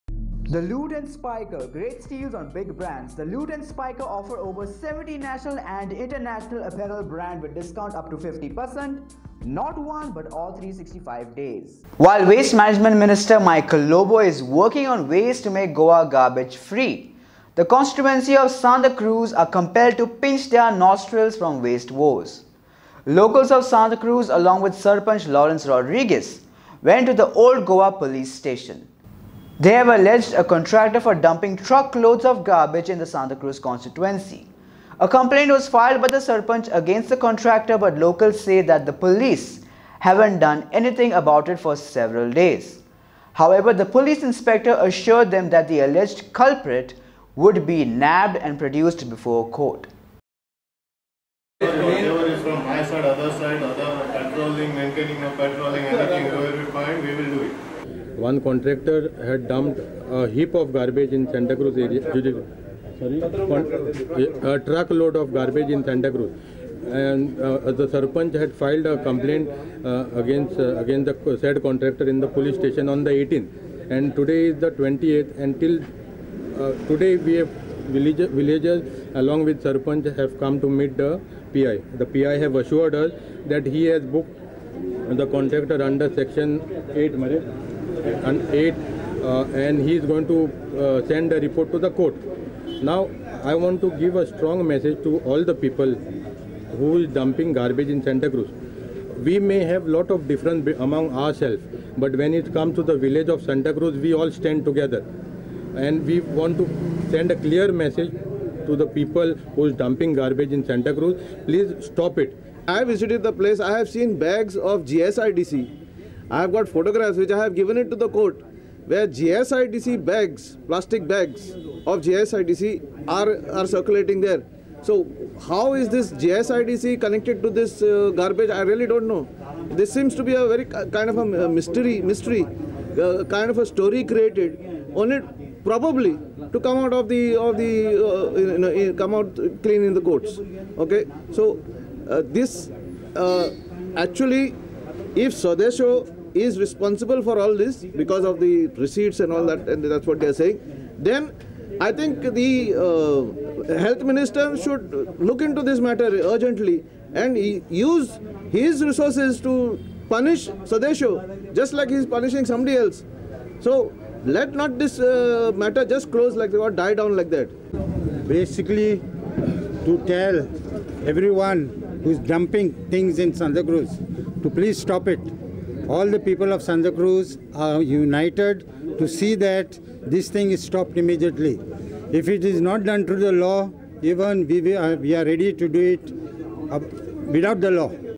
"Do Not Dump Garbage In Santa Cruz, Or Else Face The Consequences, We're Ready To Sit In Jail"
"Do Not Dump Garbage In Santa Cruz, Or Else Face The Consequences, We're Ready To Sit In Jail" - Santa Cruz Locals